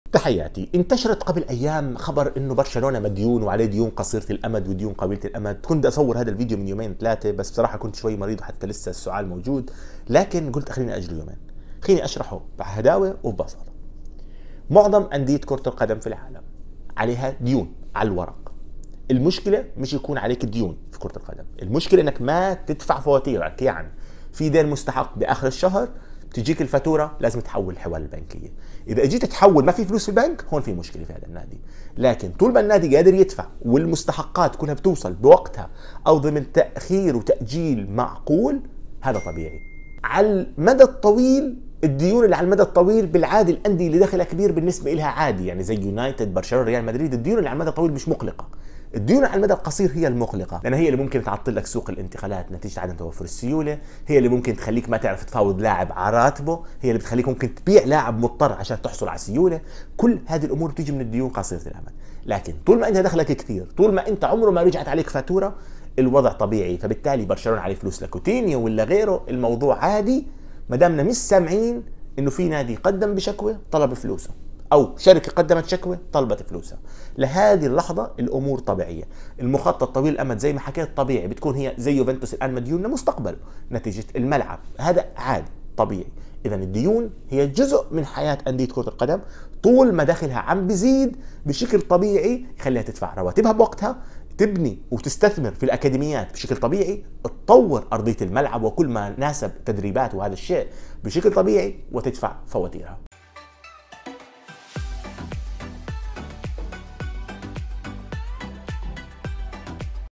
حقيقة ديون برشلونة
هل برشلونة في أزمة مالية فعلاً ؟